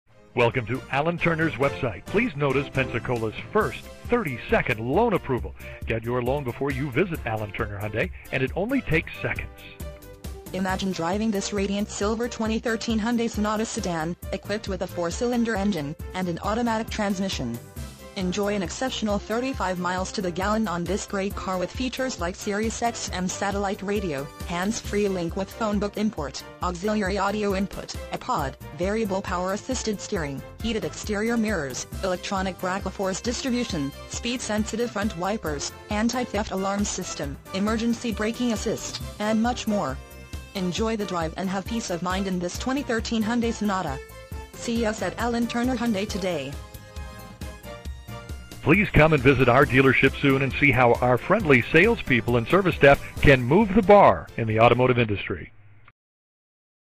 Hyundai Sonata, Allen Turner Hyundai- Pensacola, FL 32505
Hyundai Sonata, Allen Turner Hyundai You will love this Radiant Silver 20 13 Hyundai Sonata Sedan, equipped with a 4 Cyl. engine and an automatic transmission. Enjoy an exceptional 35 miles to the gallon on this great car with features like Sirius Xm Satellite Radio, Handsfreelink With Phonebook Import, Auxiliary Audio Input - Ipod, Variable Power-Assisted Steering, Heated Exterior Mirrors, Electronic Brakeforce Distribution, Speed Sensitive Front Wipers, Anti-Theft Alarm System, Emergency Braking Assist, and much more. Enjoy the drive and have peace of mind in this 20 13 Hyundai Sonata. See us at Allen Turner Hyundai today!

miles-      3

vin- 5NPEB4AC3DH738464

6000 Pensacola Blvd.